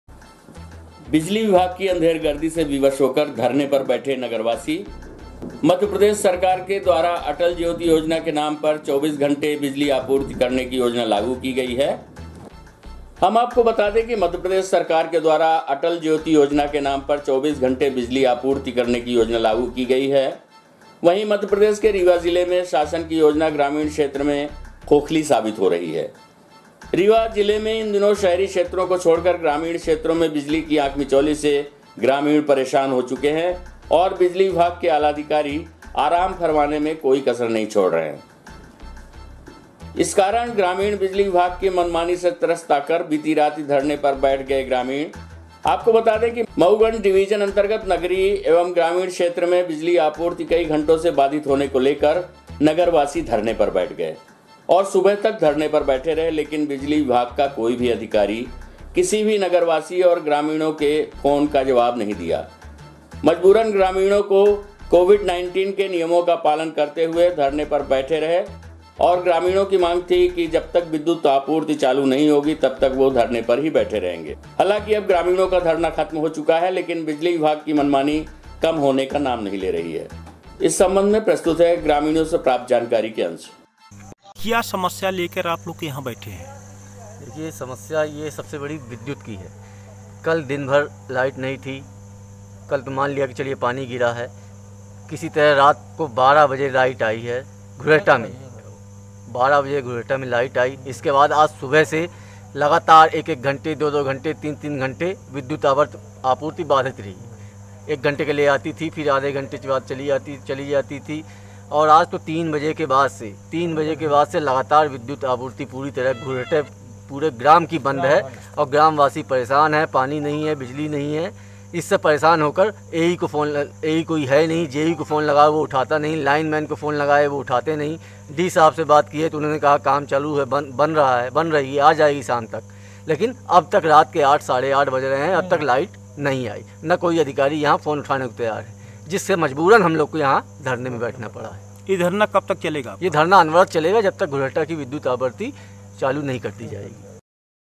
बारिश के चलते मऊगंज में बिजली आपूर्ति हुई ठप्प,, विद्युत कार्यालय के बाहर धरने पर बैठे नगरवासी,,देखे
*बारिश के चलते मऊगंज में बिजली आपूर्ति हुई ठप्प,, विद्युत कार्यालय के बाहर धरने पर बैठे नगरवासी,,,,देखें सिटी न्यूज़ वैन की ये खबर*